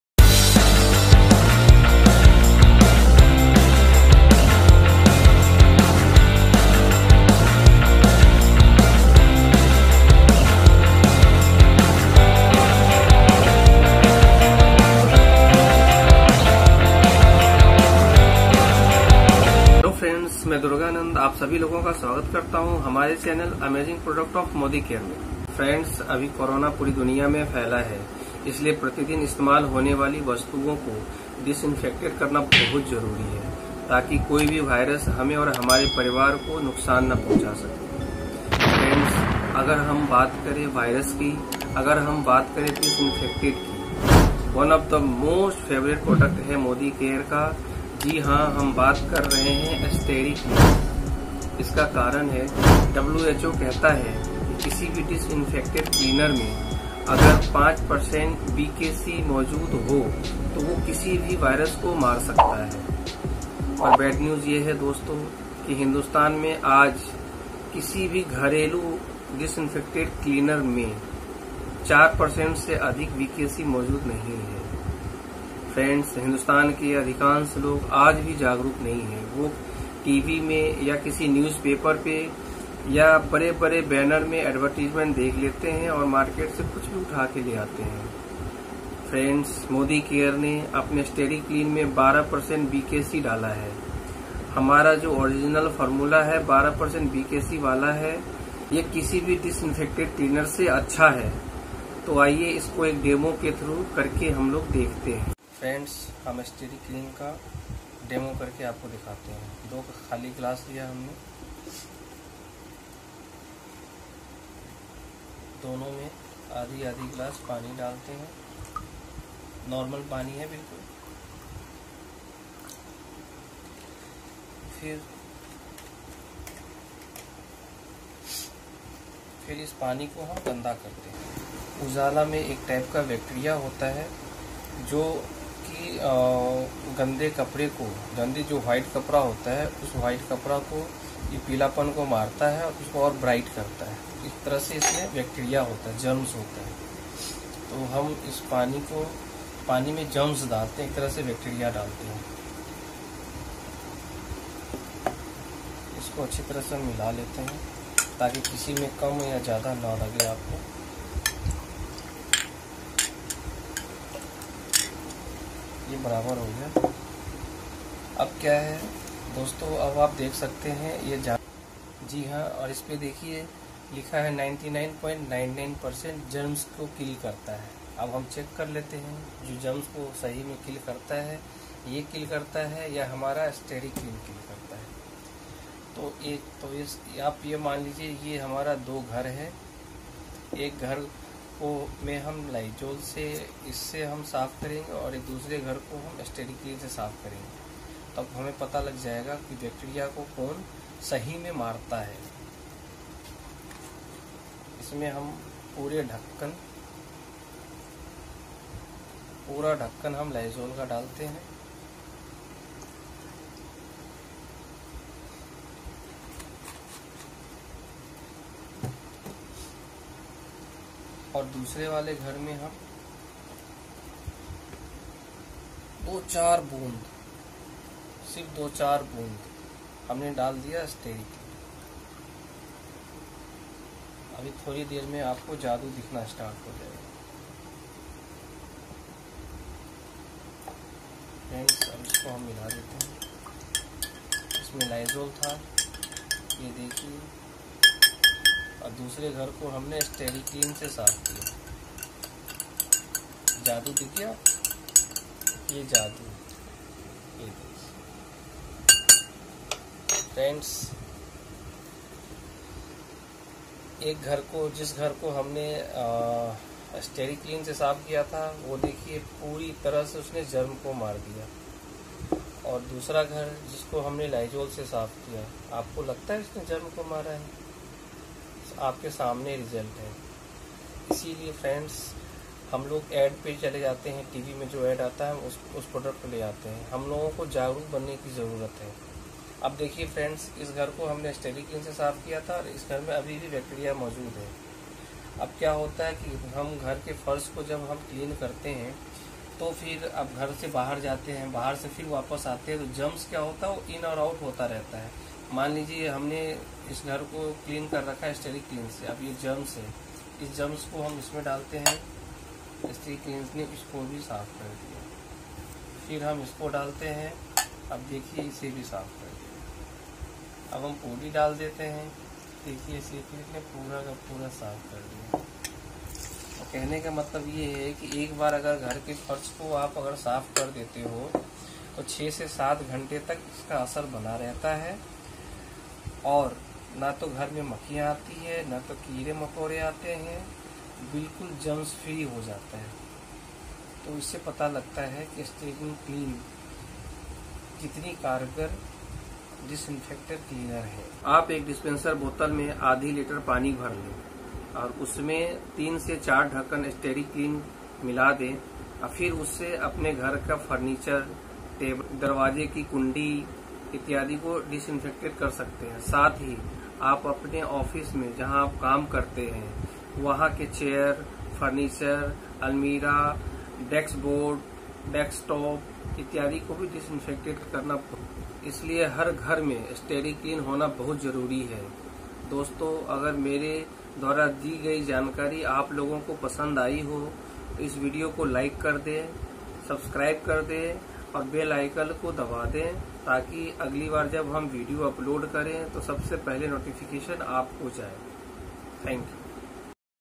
100% Result on Stericlean Demo Disinfectant Cleaner with Modicare Product को समझे
100% Result on Disinfectant Cleaner with Modicare Product compare with market products